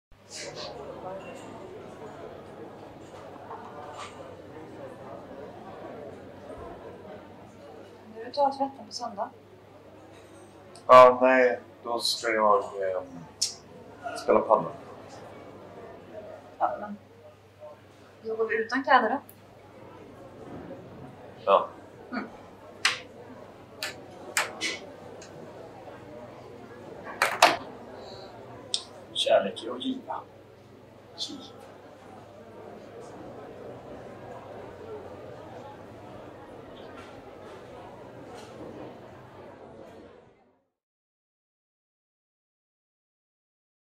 Krogbesök med gnissel
I behov av teaterterapi? Karaktärerna i Strindbergs "Fadren" spelar sina roller i en nutida miljö.